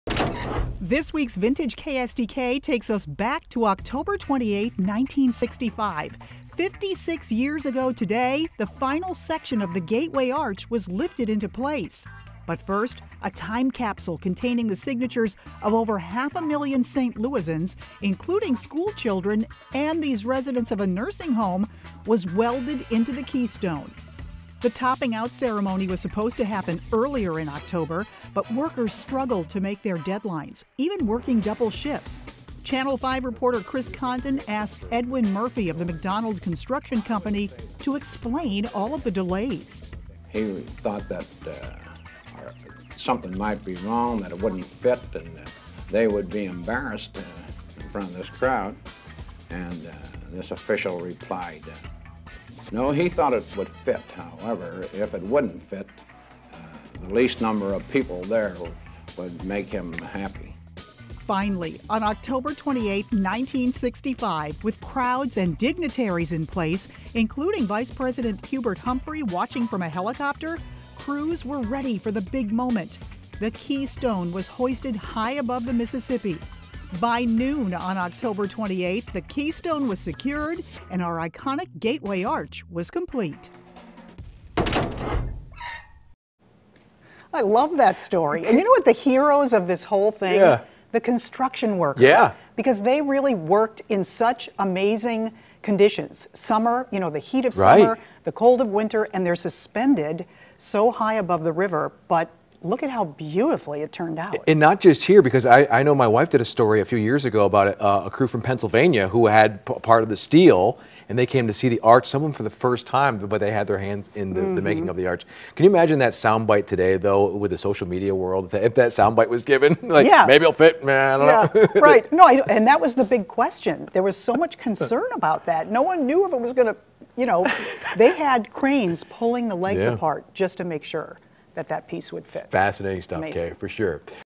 Vintage KSDK: Gateway Arch is completed in 1965
This week's Vintage KSDK takes us back to October 28, 1965. Fifty-six years ago, the final section of the Gateway Arch was lifted into place.